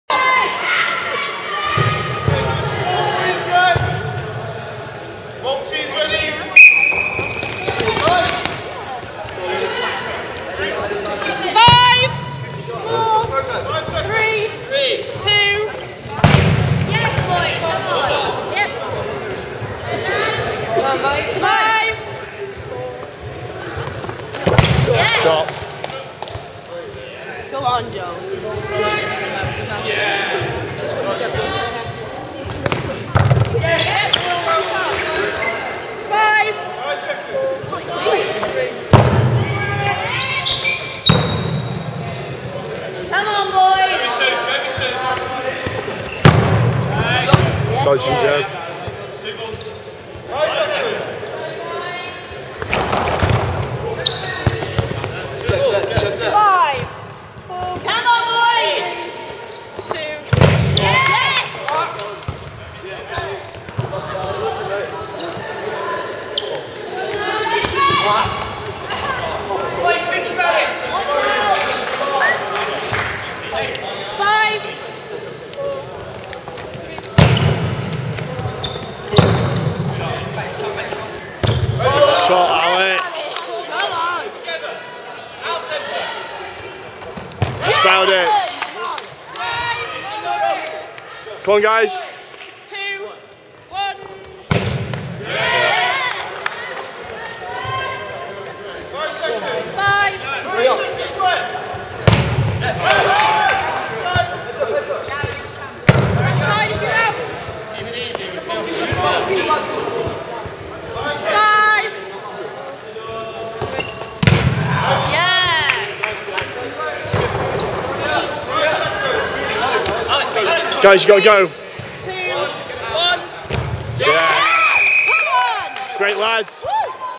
Warwick Uni Warriors I vs UoB Bedford Bulls I - Uni Cup Finals - Final - Game 1
Warwick Uni Warriors I (Red) vs UoB Bedford Bulls I (White) in the Finals of the University Cup Finals 2011.

Pre-game Score: Warwick 0 - 0 Bedford